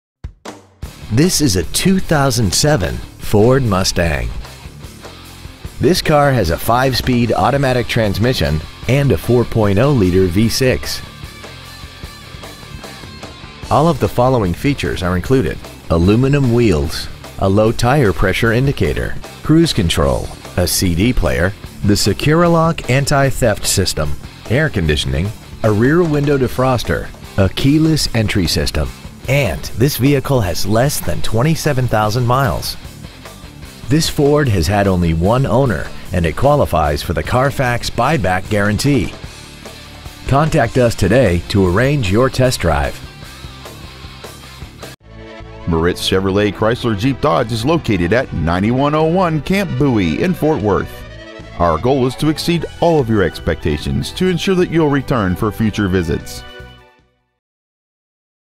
2007 Ford Mustang Arlington TX
Year : 2007

 Make : Ford

 Model : Mustang

 Engine : Gas V6 4.0L/244

 Trans . : Automatic

 Exterior : Torch Red

 Miles : 26,355

 Interior : Black

 9101 Camp Bowie

 2007 Ford Mustang Fort Worth TX